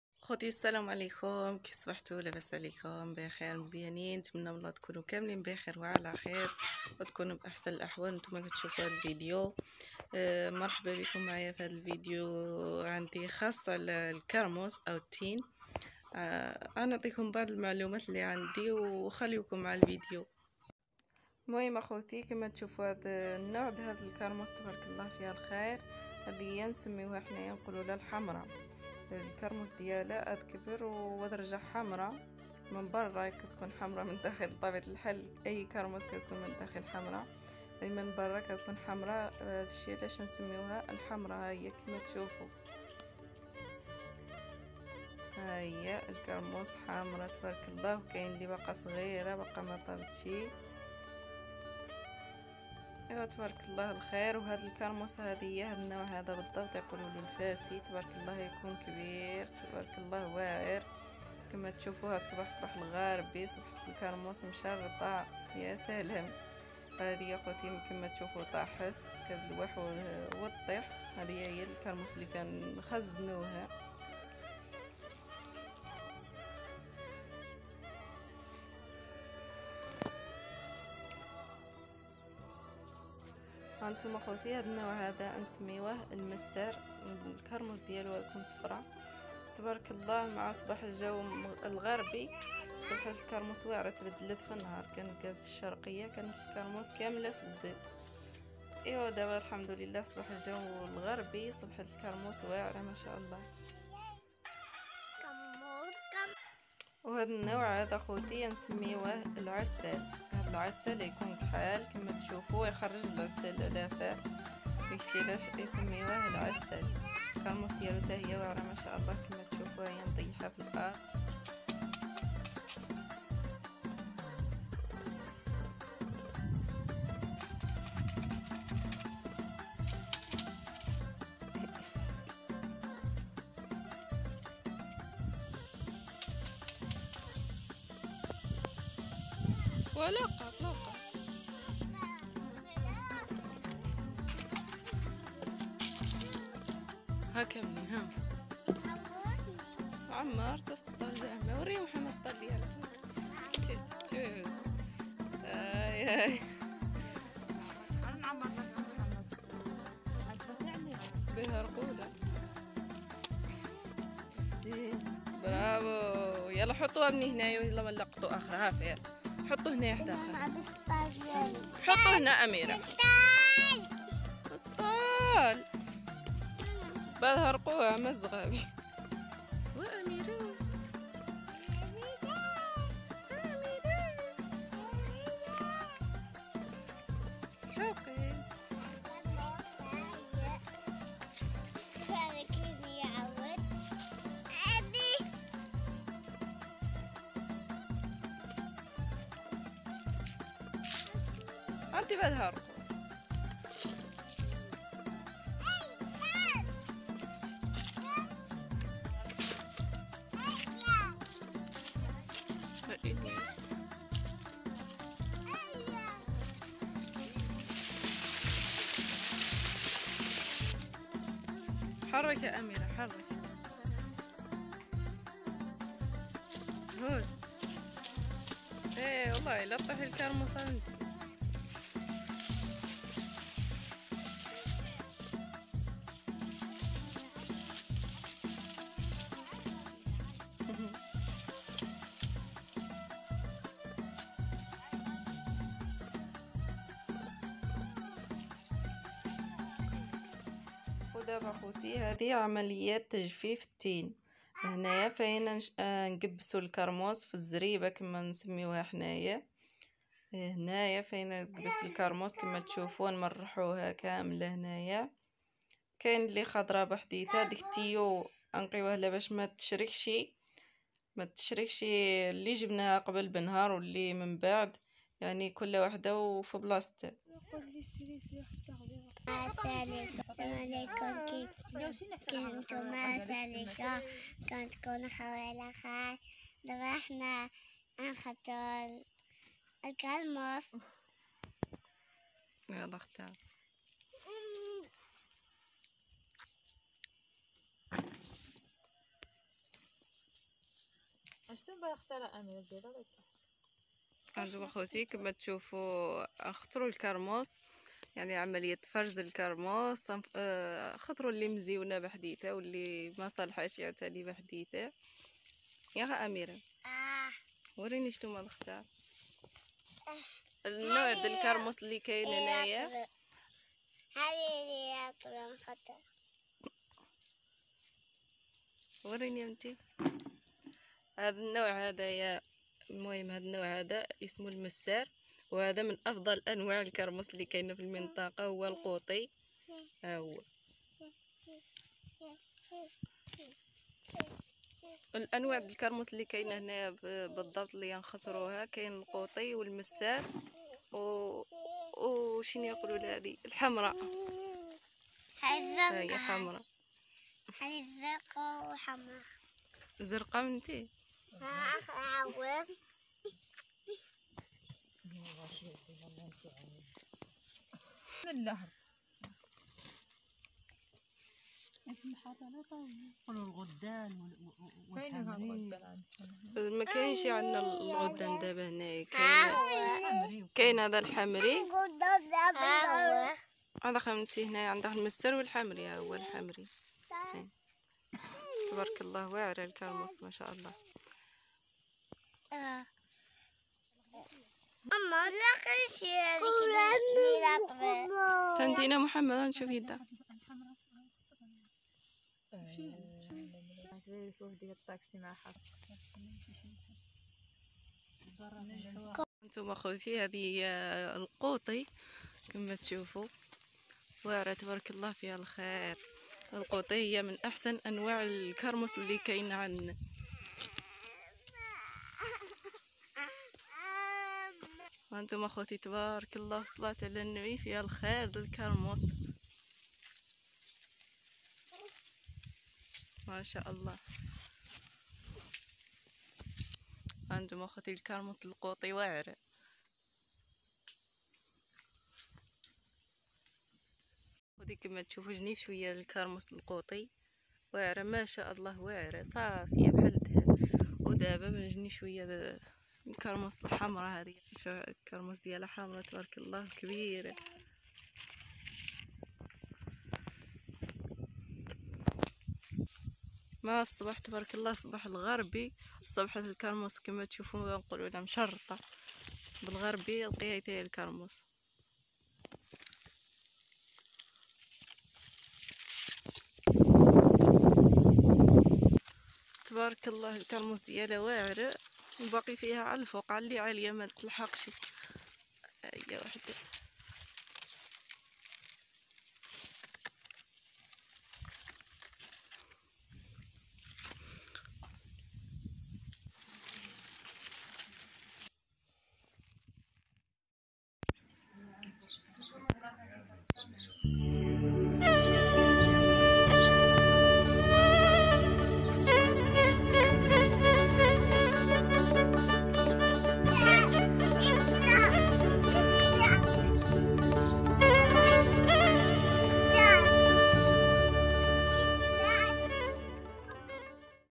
شاهد مراحل تجفيف وفرز تين أو كرموس في بني أحمد شفشاون
قال تعالى (واتين و الزيتون واطوري سنين وهذا البلاد الأمين)

اللهم صلي وسلم وبارك على سيدنا محمد وعلى آله وصحبه وسلم تسليما كثيرا